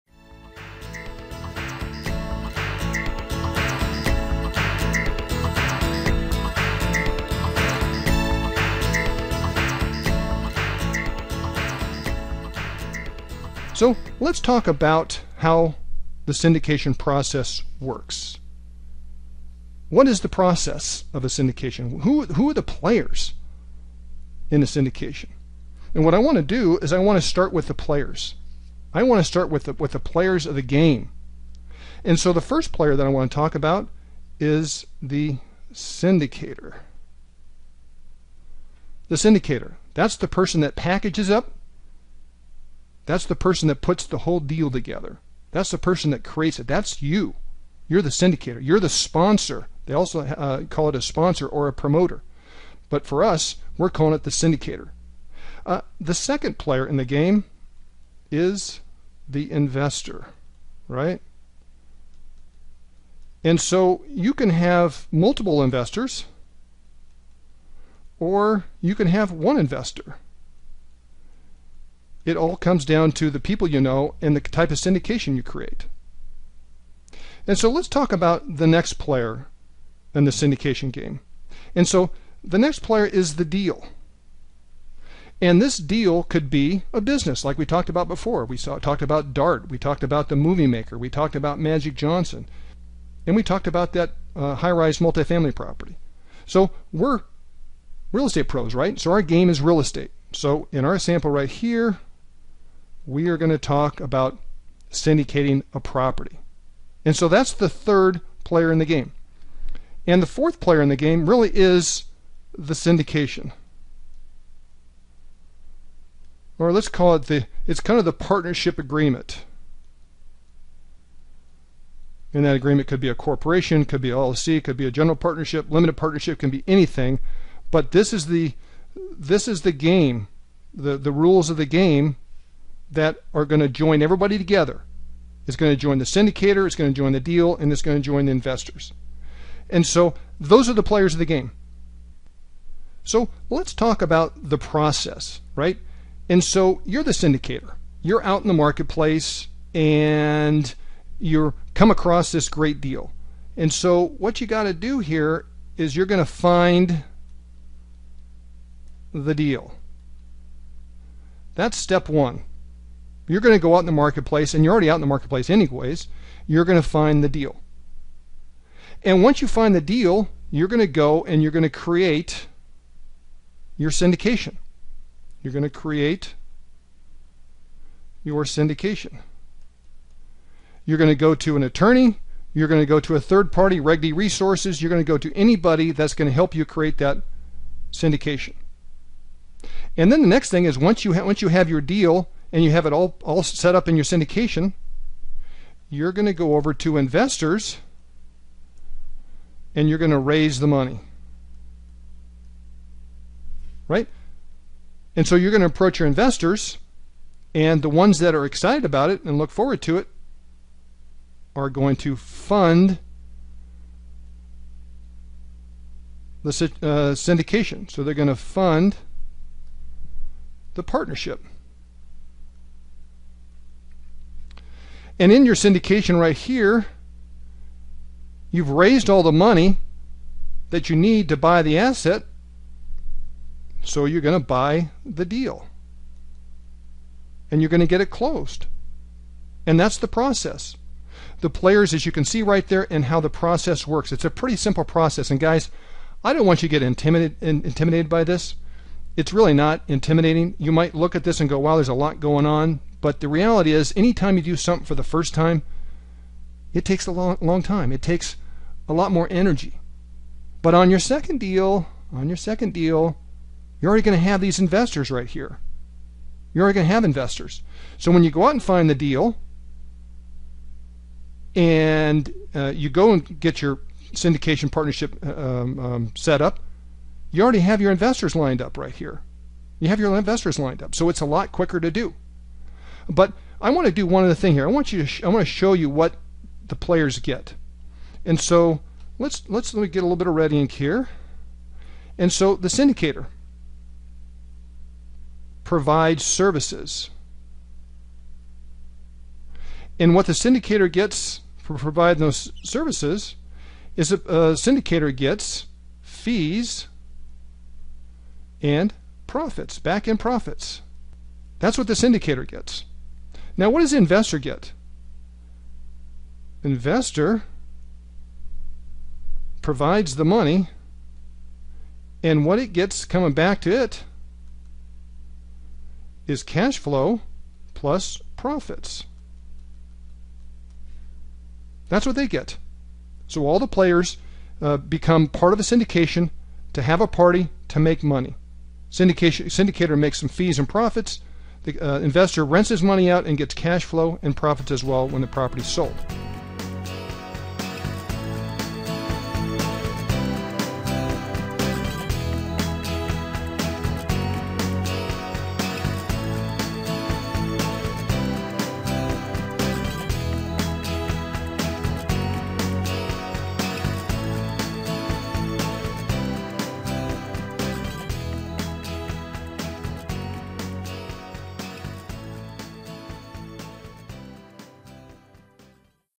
How Syndication Works Video
Check out my short video, How Syndication Works where I explain the roles of the players in this money game.

WANT MORE FREE RESOURCES?  We have tons of other free articles, interviews, tutorials, books and training